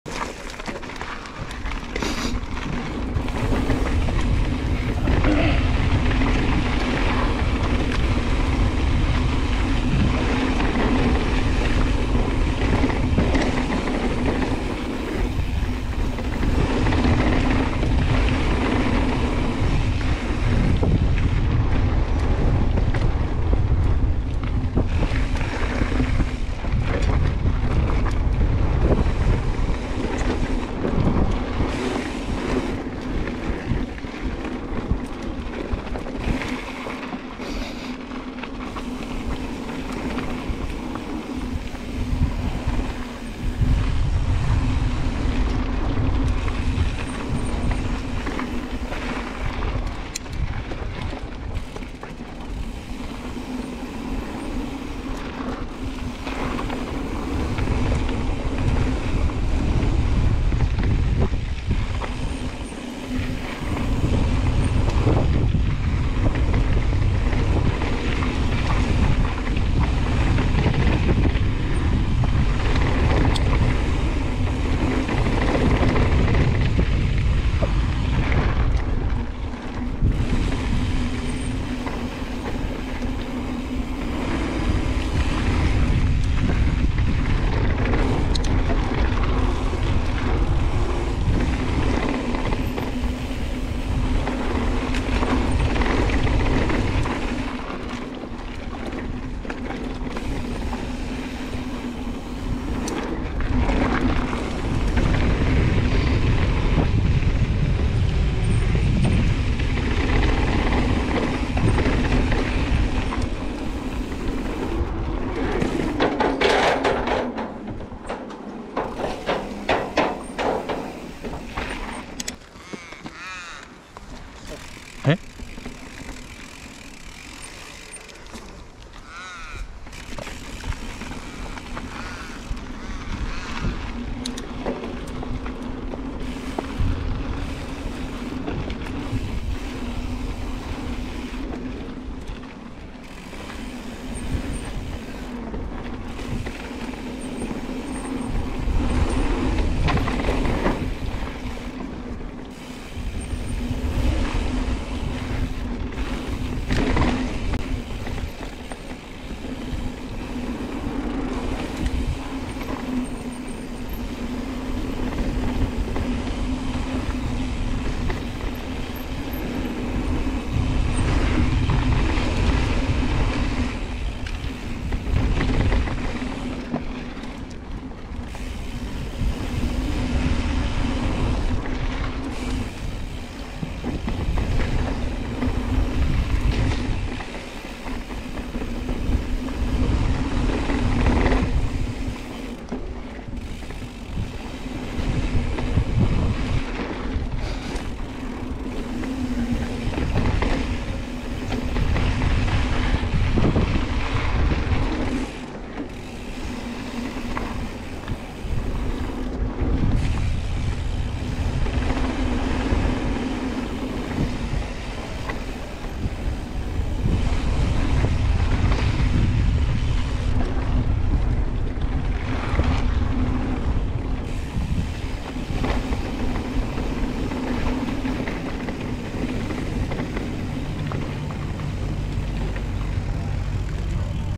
Thredbo Bike Park - Kosciuszko Flow (lower half)
Riding the lower half of blue trail "Kosciuszko Flow" at Thredbo Bike Park, in NSW Australia.